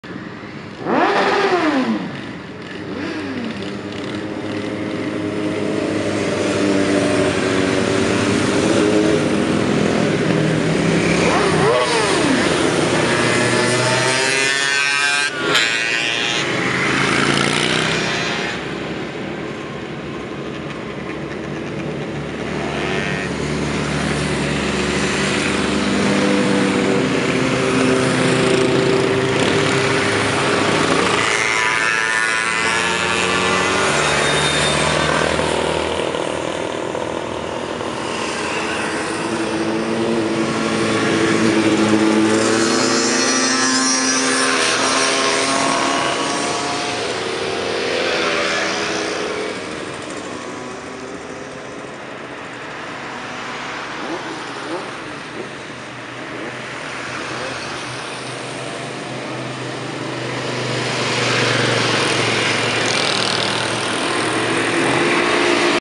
Rozpoczęcie sezonu motocyklowego- przejazd ul. Z. Augusta cz.2
Limanowa: Rozpoczęcie sezonu motocyklowego i motorowerowego 2016. Przejazd kolumny motocykli przez ulicę Zygmunta Augusta. cz 2